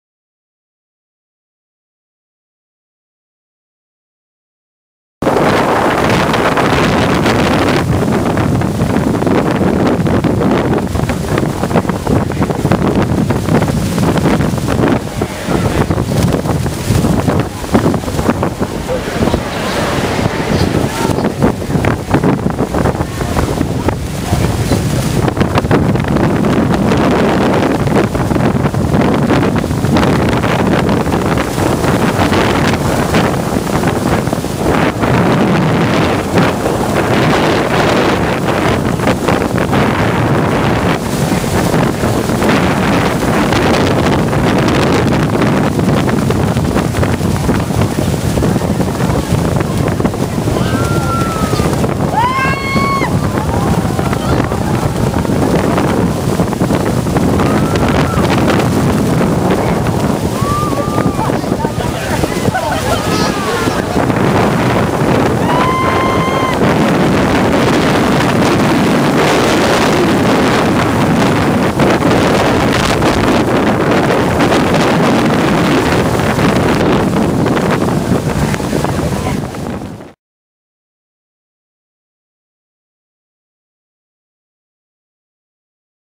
Niagara Falls Up Close - Maid of the Mist (Feel the Power of Niagara in HD)
Please Subsribe!

Journey into the heart of Niagara Falls via the Maid of the Mist boat ride.  Taken on the top deck of the Maid of the Mist, this video lets you feel the immense power of the Niagara Falls.  It is like being in the eye of a hurricane.  You will be wet, very wet if you are on the top deck.  We would do this again in swim trunks and no rain jackets!  It is thrilling and fun for the summer months!